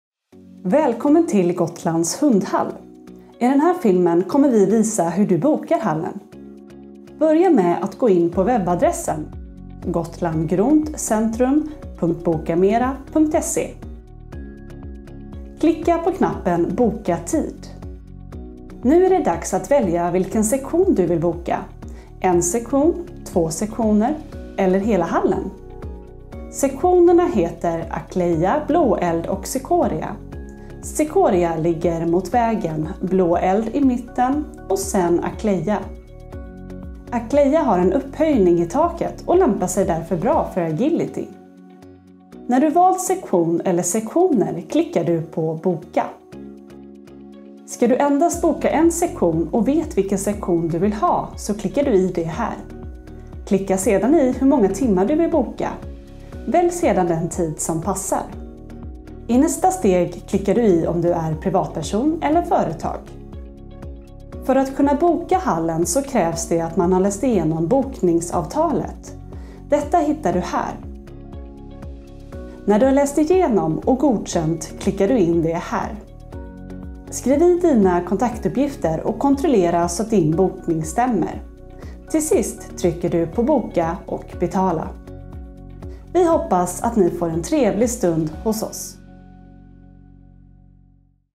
Gotlands Hundhall – Såhär bokar du!
Välkommen till Gotlands nya inomhushall för hundträning – en efterlängtad plats för tävlingar, kurser och träning, perfekt för dig
och din hund.

I den här filmen går vi igenom hur man bokar hundhallen

Om Gotlands Hundhall:
Uppvärmd året runt * Över 900 kvadratmeter yta * Konstgräs och sandunderlag * Hyr hela hallen eller bara sektioner – flexibiliteten är din!

Oavsett om du är en privatperson, förening eller organisation – alla är välkomna! Boka nu och ge din hund den bästa träningsupplevelsen!